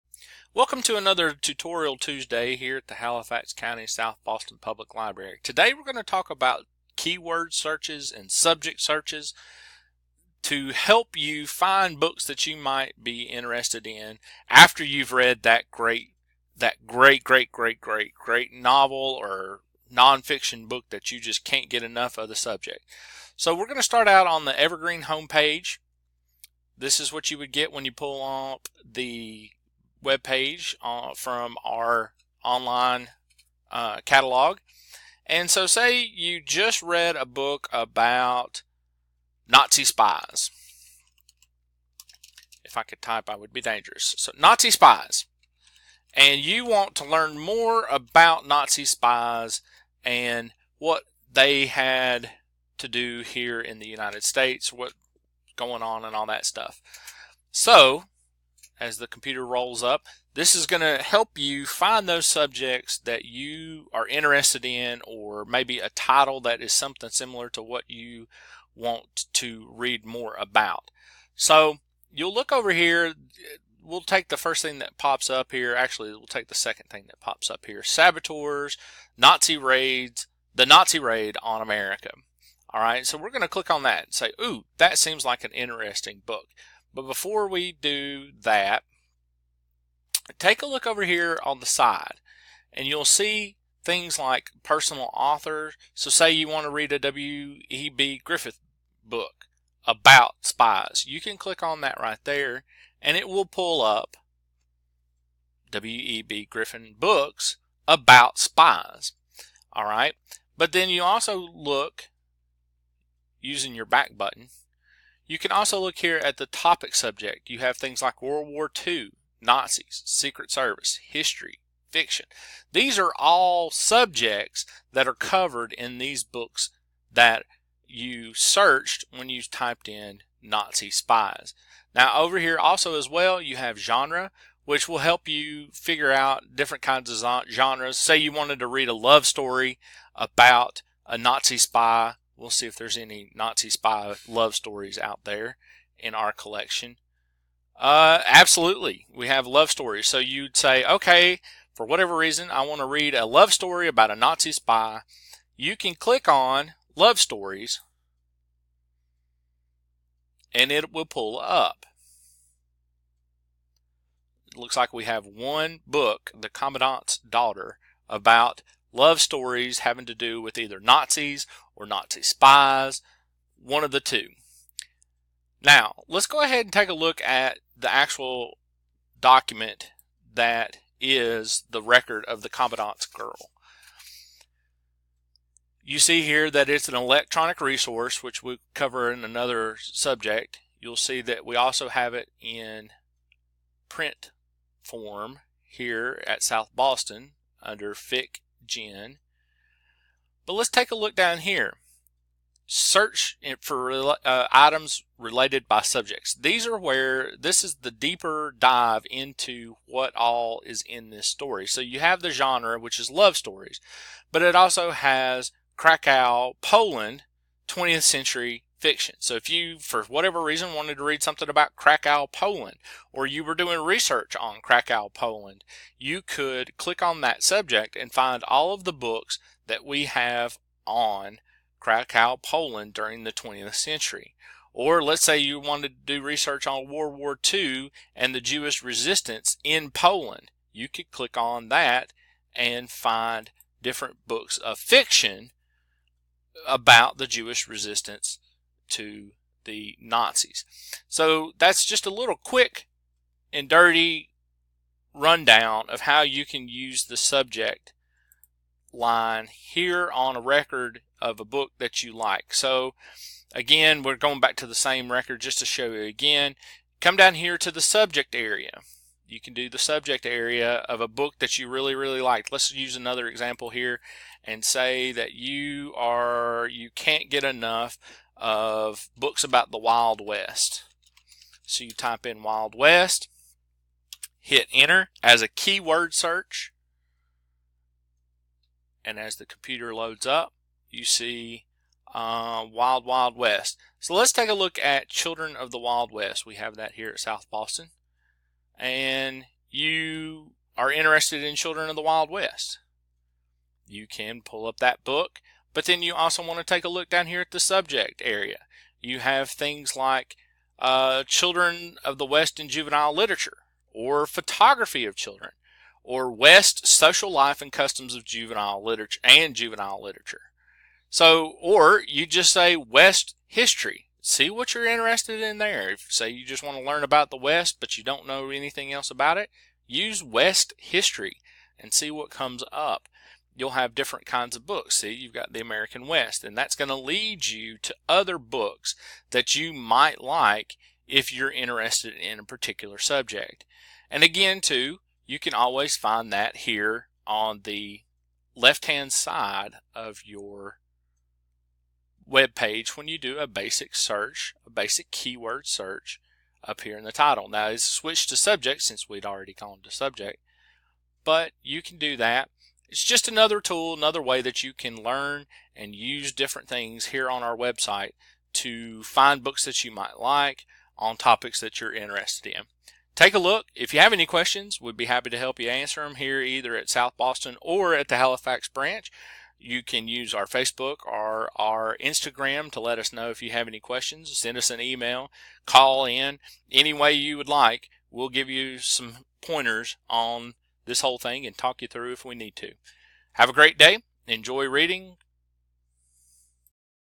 How To: Subject Search
Love reading about a certain subject and want to find every book you can about it?
Use a subject search!